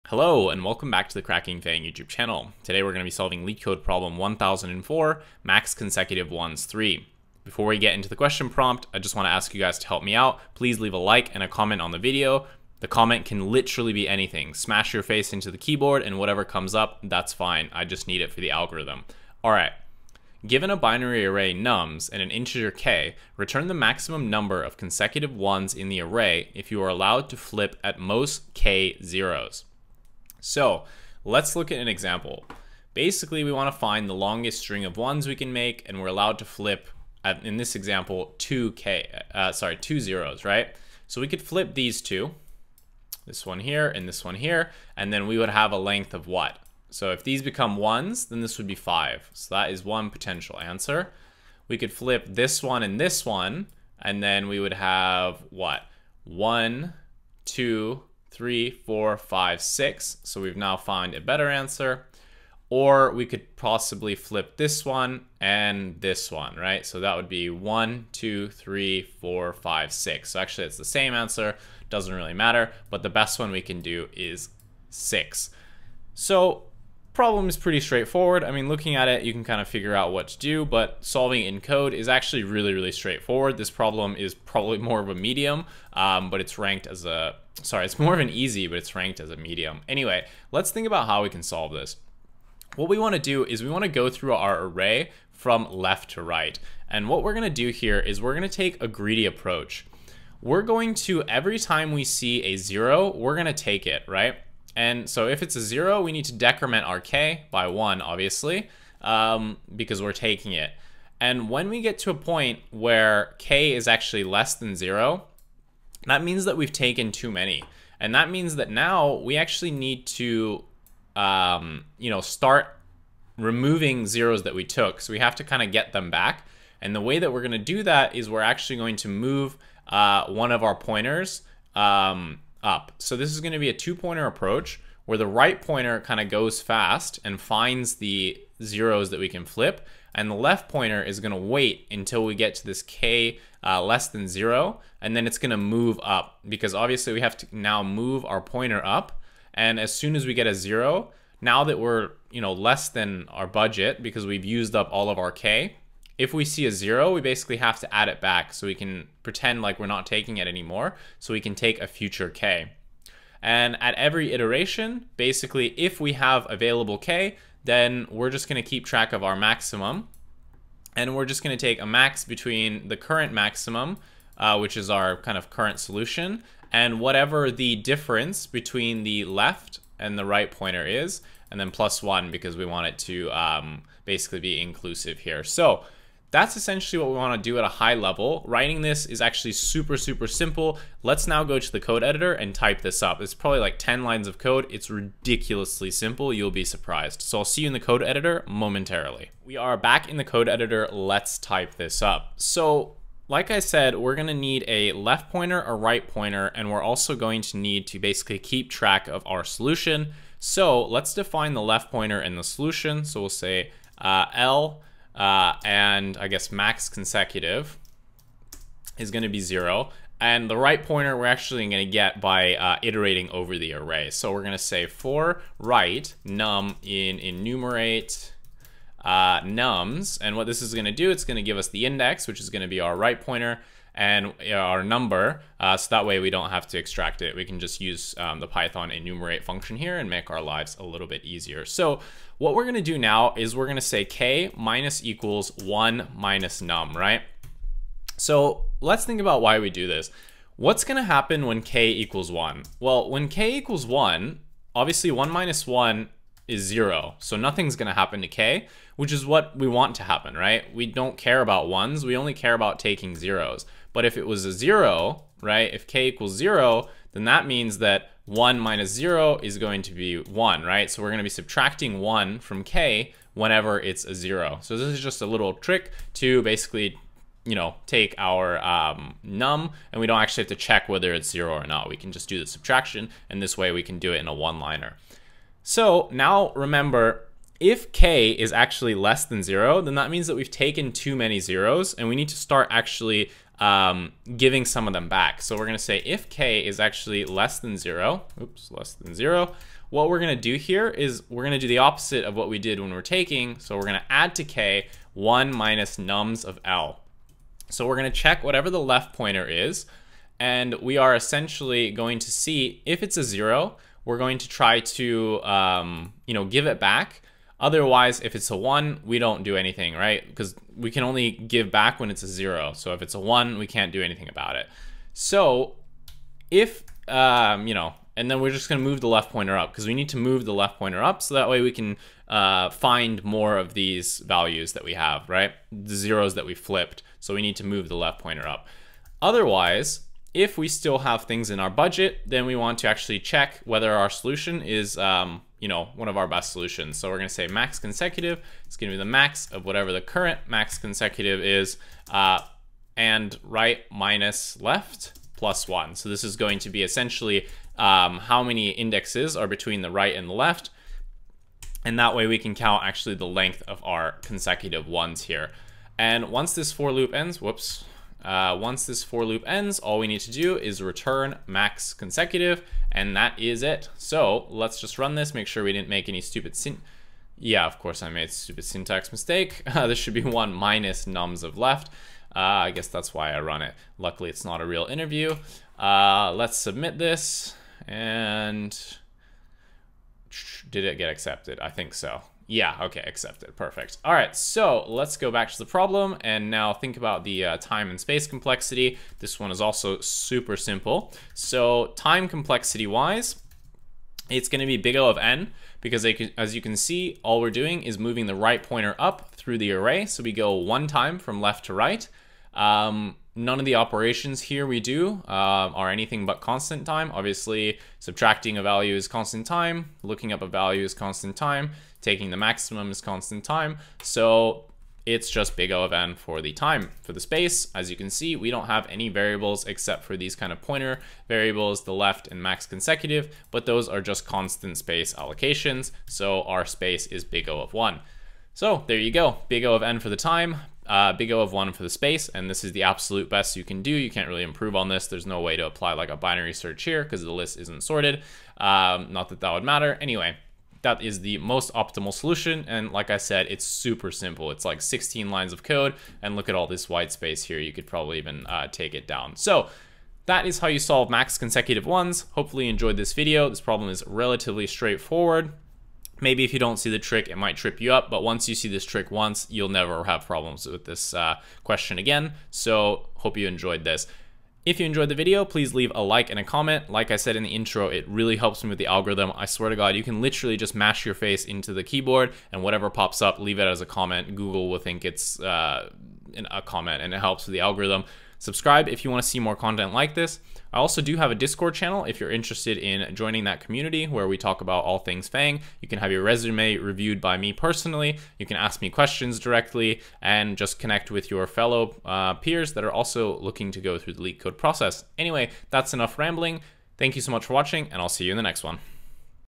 MAX CONSECUTIVE ONES III | LEETCODE 1004 | PYTHON SOLUTION
In this video we are solving a simple Facebook/Google interview question dealing with arrays: Max Consecutive Ones III (Leetcode 1004).

This question is marked as a medium but it's really more of an easy and it's super straightforward to solve.

TIMESTAMPS:
00:00 Intro
00:20 Question Prompt
00:30 Example & Solution Intuition
03:19 Coding
06:36 Time/Space Complexity
07:45 Outro